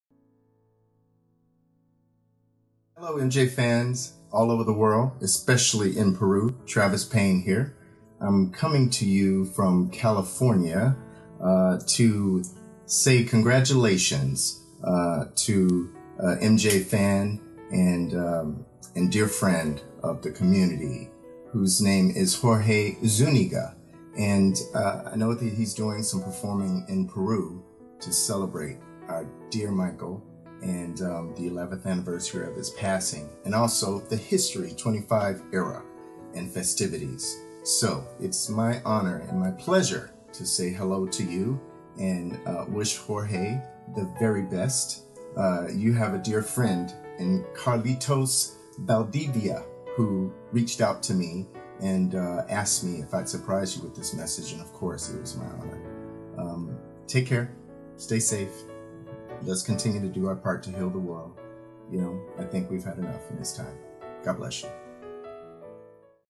Saludos de Travis Payne - Coreógrafo de Michael Jackson
Este año han habido muchas emociones, positivas y negativas, igualmente el camino de la vida es tan complicado pero aquí nos encontramos todos luchando por algún objetivo, a pesar que a veces sintamos que el mundo se nos venga encima.

Siempre he sido creyente de hacer todo de corazón, de buena fe, de caerme y seguir levantándome así esté agonizando, el tiempo poco a poco te va haciendo entender algunas cosas y a la vez poniendo todo en su lugar.

Recibir una sorpresa de esta magnitud no tiene precio, de una persona que ha trabajado durante años con mi artista favorito, teniéndolo muy de cerca, artista a quién le rindo tributo de todo corazón, esforzándome por mejorar cada vez más, es un trabajo de años y aún estamos en ese proceso.

No tengo como agradecer este gesto que no hubiera sido posible sin "People of the World - MJJ Perú" (@pwmmjperu), comandada por Carlitos Eduardo, quién me hizo una sorpresa y en verdad, ante cualquier cosa, no tengo palabras.

Ya le hice llegar mi saludo al buen Travis, amigo, bailarín y coreógrafo de Michael Jackson durante años. Vengo haciendo una lucha de años, aprendiendo pese a todo, ponerte en los zapatos de Jackson es super difícil pero lo disfruto tanto y soy tan feliz haciéndolo que nada ni nadie va a poder quitarme ese sentir. Aprecio tanto el mensaje y la buena vibra de este señor que estoy saltando de alegría.

Gracias a Marcela S. y Adriana C., dos buenas amigas que me ayudaron a traducir de la mejor manera lo que Travis comentaba en el video.
Gracias a todos nuevamente.